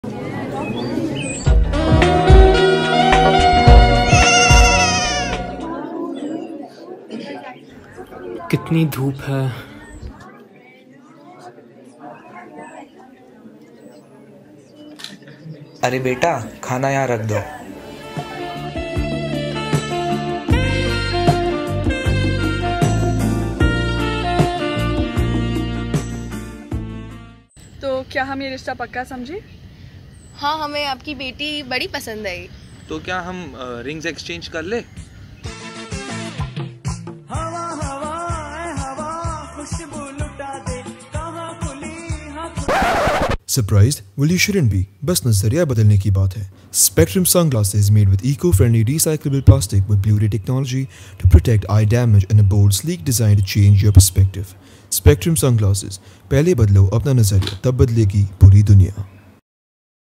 Sunglass Ad film - Spectrum Sunglasses
"Be the change you want to see in the world" and if that seems like too much effort, simply - change the way you see the world. At the end of the day, Nazariye ki baat hai!

Cast:
Shreay Shah
Pooja Meswani
Nishma Desai
Suraj Prakash
Samarth Shah
Sarvath Shetty
Sattwik Mohanty
Arshit Shetty

Script:
Simran Raina

Creative direction:
Drishti Shah
Sattwik Mohanty
Samyak Poddar
Simran Raina
Shreay Shah
Pooja Meswani
Namrata Chawla

Editing:
Omansh Agarwal
Tanay Sanghvi

Cinematography:
Tanay Sanghvi